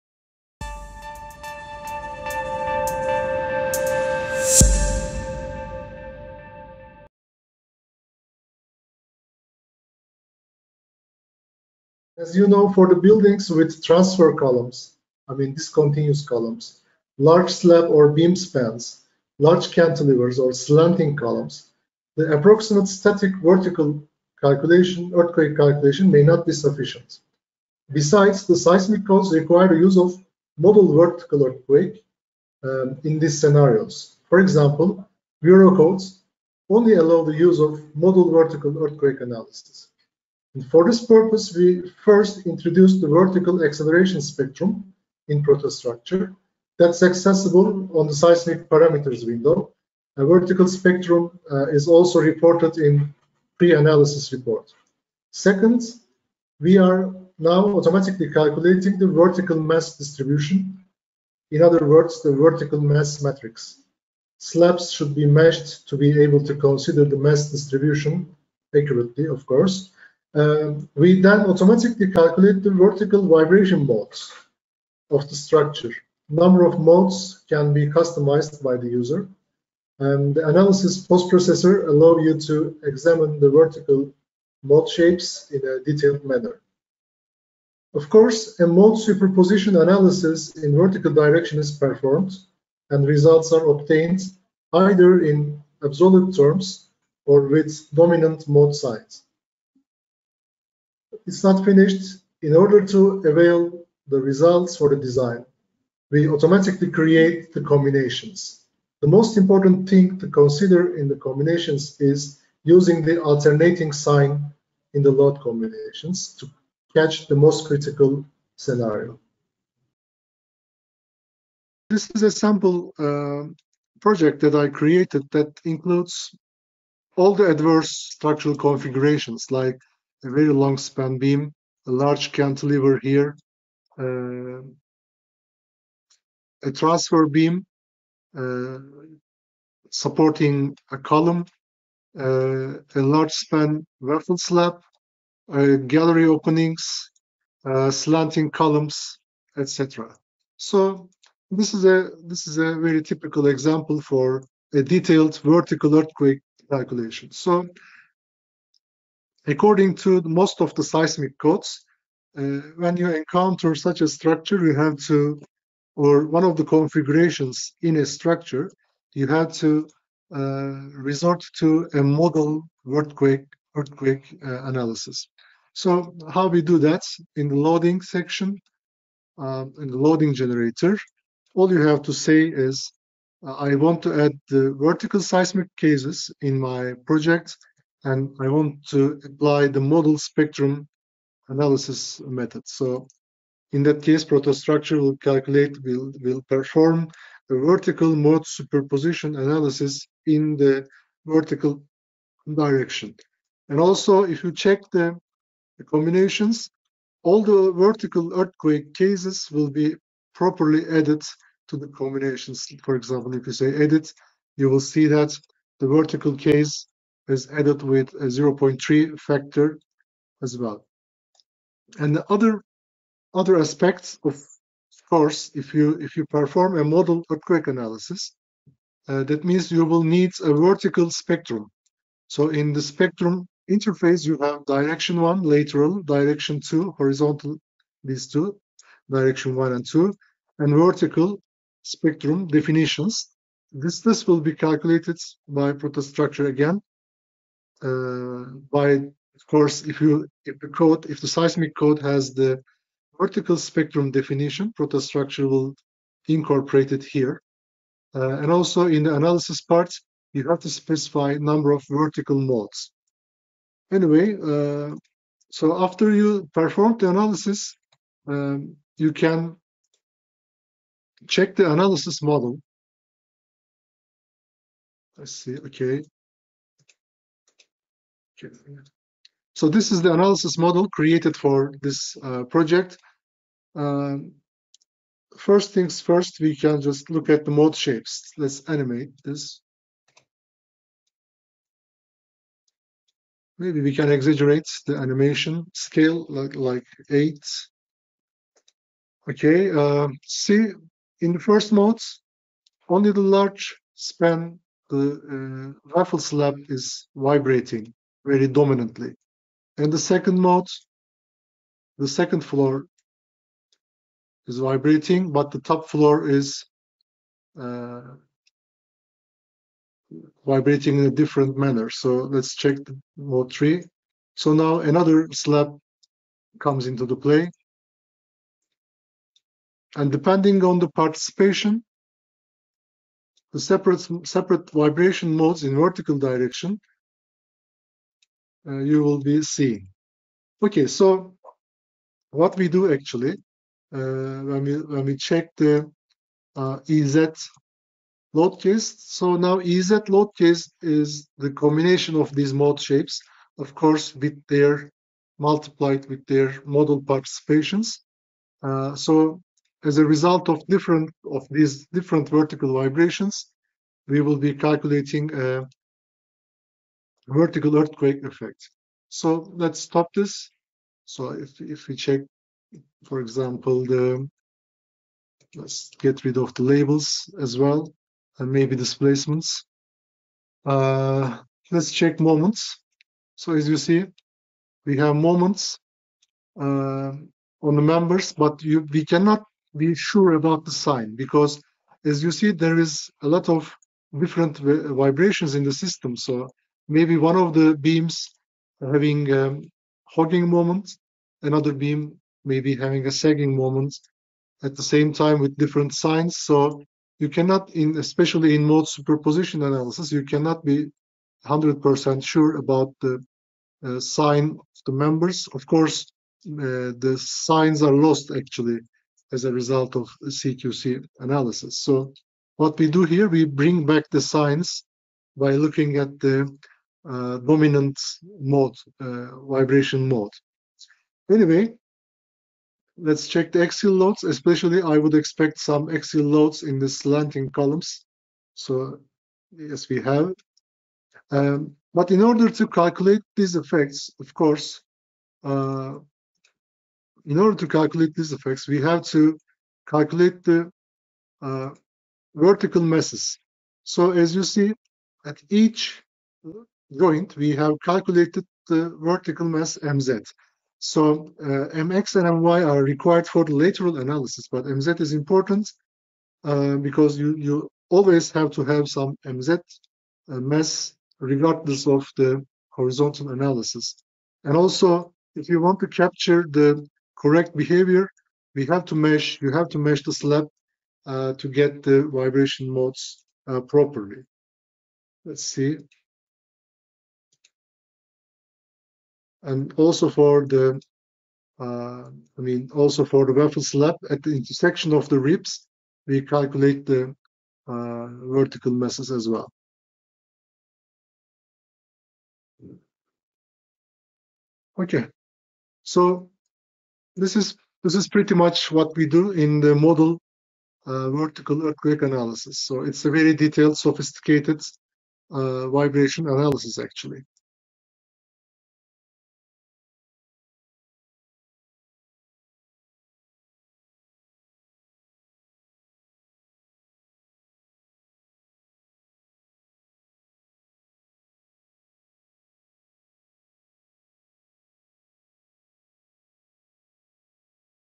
Modal Vertical Earthquake Analysis with the ProtaStructure 2025
With its new features for advanced earthquake engineering applications, ProtaStructure now enables you to perform modal vertical earthquake analyses. ProtaStructure reflects the effects of vertical earthquake action to design by following two methods:
• Approximate Static Approach
• Modal Spectrum Analysis Method

In the Approximate Static Approach, vertical vibration modes and a dedicated vertical spectrum are not considered. Instead of those, the result from gravity load case is multiplied by a fraction of horizontal spectral acceleration. On the other hand, in Modal Spectrum Analysis Method, the vertical vibration modes of the structure are considered together with a vertical acceleration spectrum.

In this video, Prota Software Product Manager Mustafa Tan demonstrates how to perform the modal vertical earthquake analysis with ProtaStructure through a live demonstration. Moreover, he shows how to visualize the analysis results on the analytical model.

The all-in-one structural engineering software, ProtaStructure Suite maximizes your efficiency in structural modeling, analysis, design, detailing and fabrication processes.